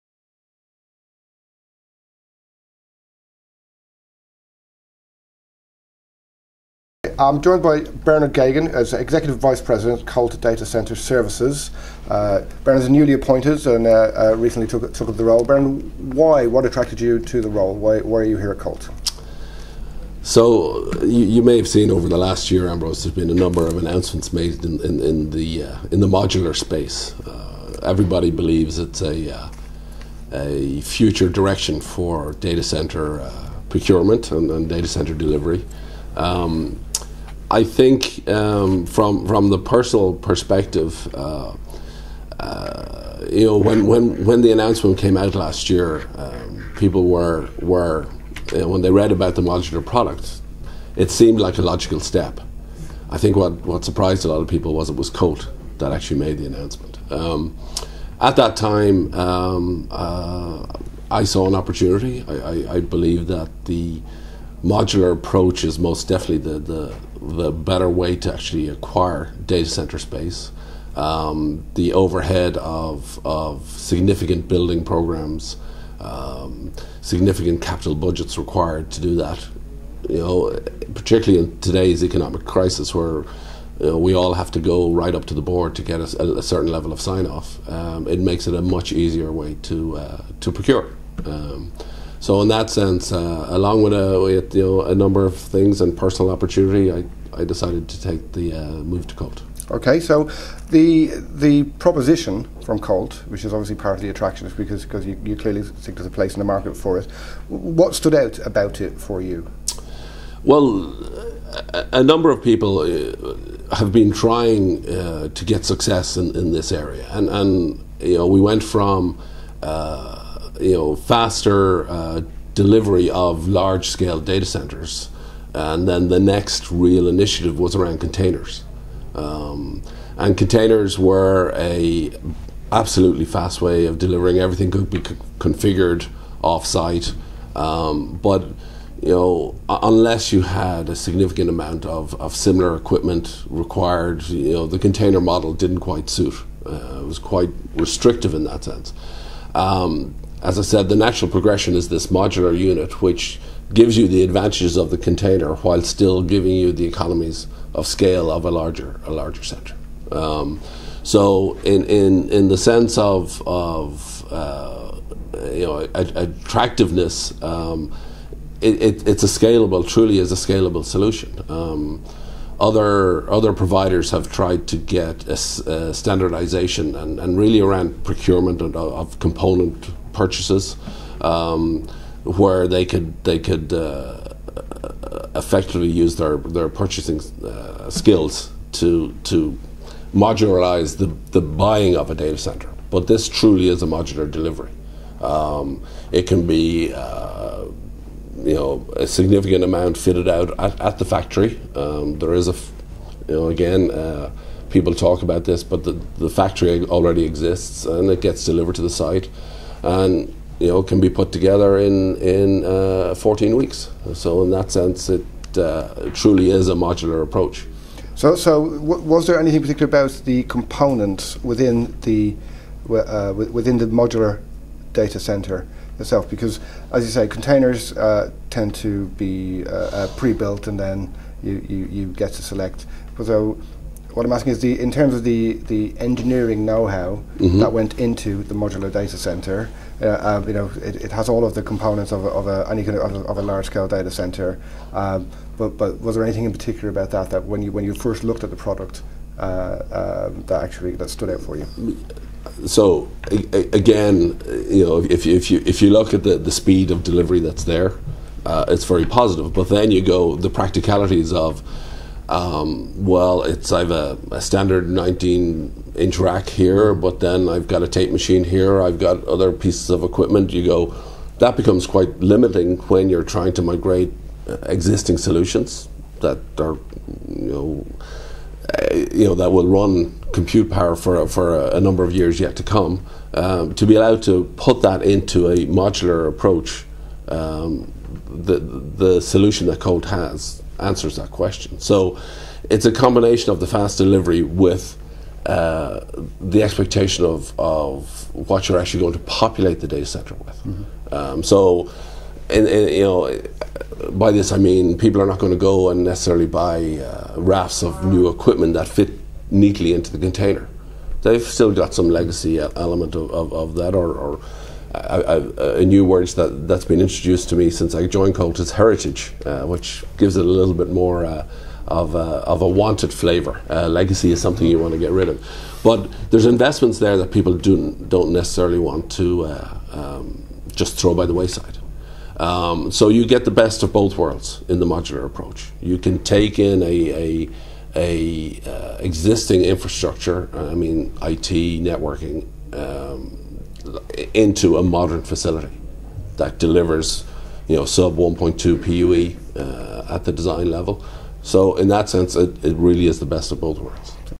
Colt's Modular Data Centre
DatacenterDynamics talks to Bernard Geoghegan, recently appointed Executive Vice President of Colt Data Centre Services, about his role and how he sees the company develop their offerings to the market